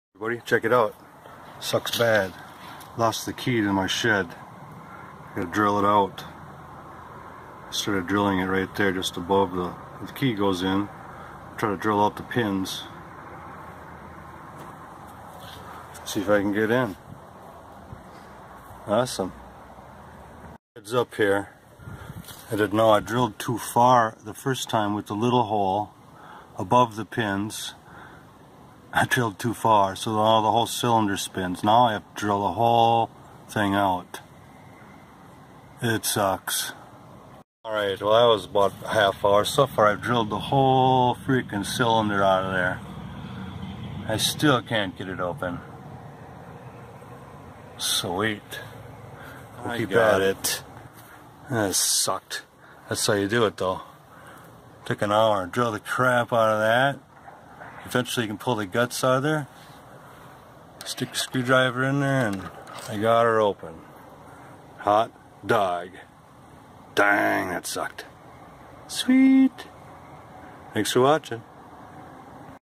LOST THE KEY TO THE SHED, THIS IS WHAT GOES DOWN!
Lost the key to the shed so I had to drill it out!